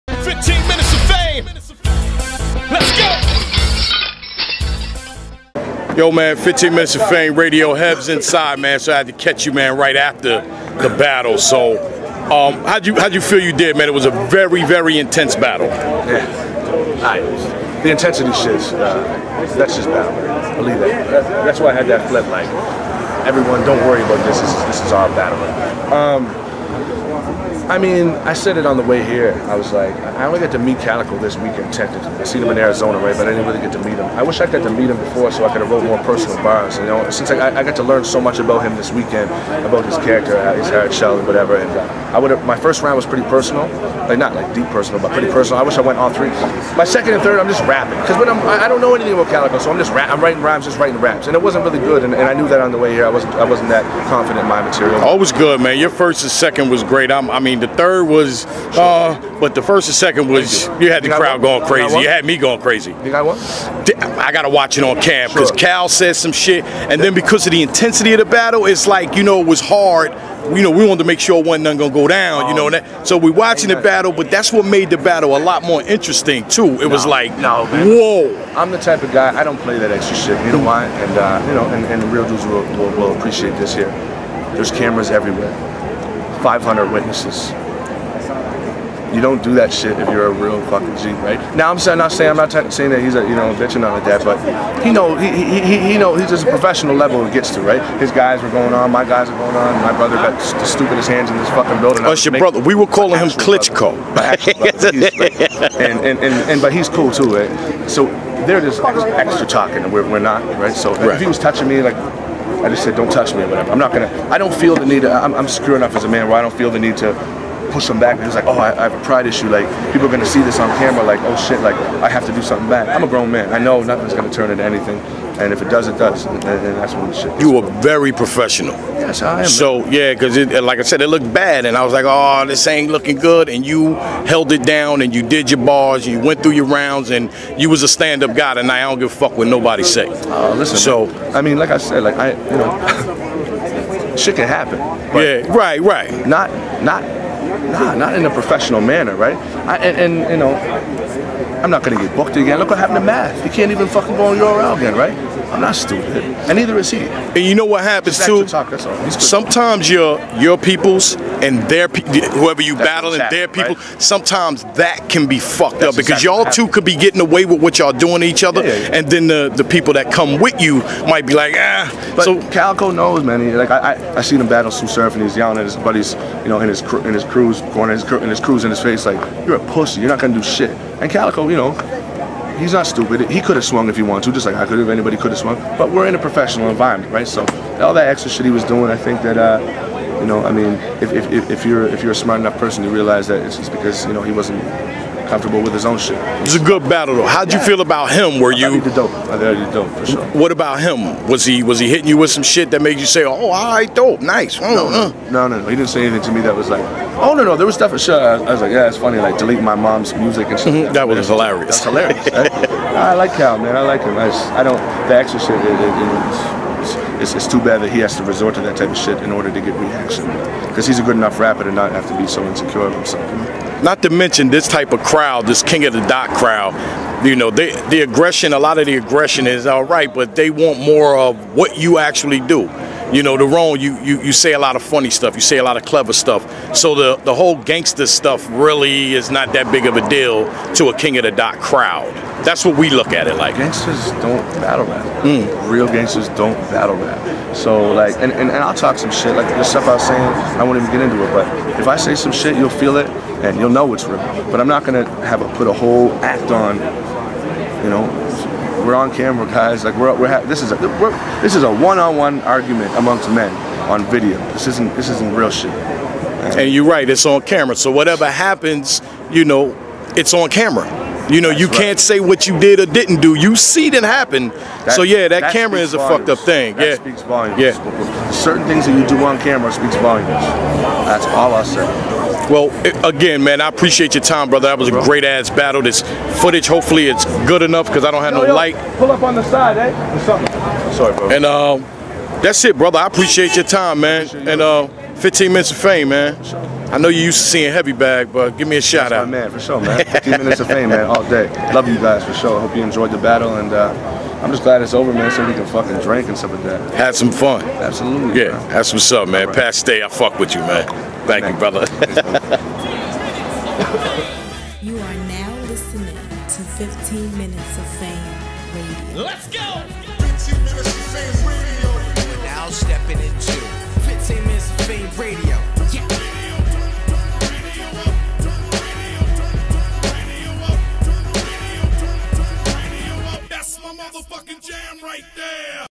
PAT STAY ON CALICOE'S ACTIONS AT KING OF THE DOT BATTLE & RECAP
Live Show Lineup:
Wednesday: Charlie Wilson Radio - 9-11pm (EST)
Thursday: The Heavy Bag Show 8-10pm (EST)
Sunday: Judgment Day Show - 8-10pm